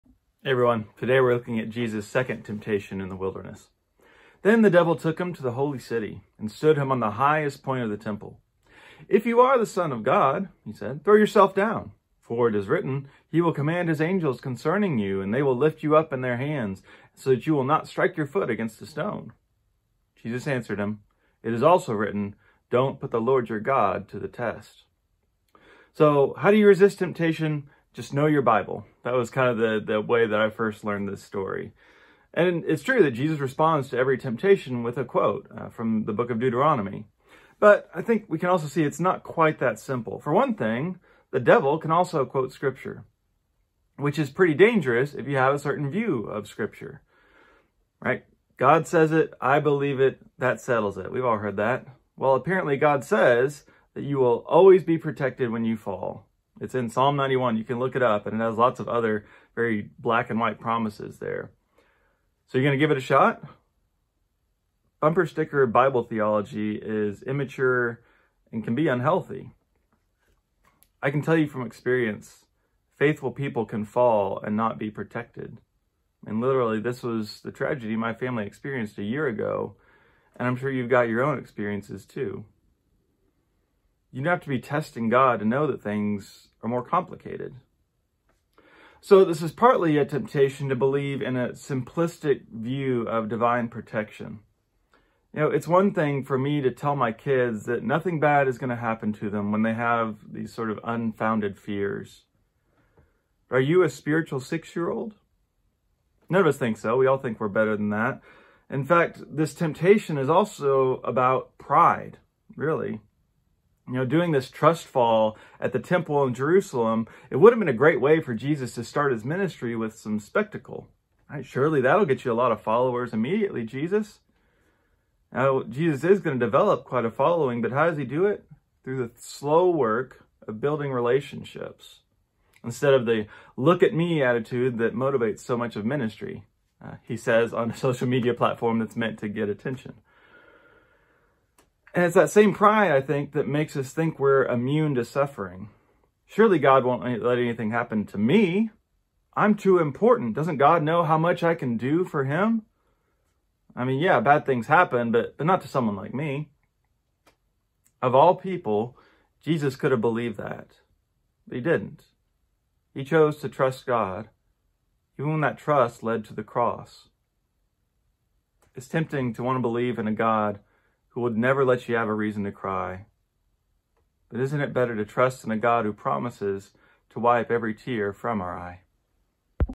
2nd Temptation: Pride (Matt 4:5-7)
Chris’s thought for Feb. 24 on simplistic perspectives on Scripture and pride about divine protection.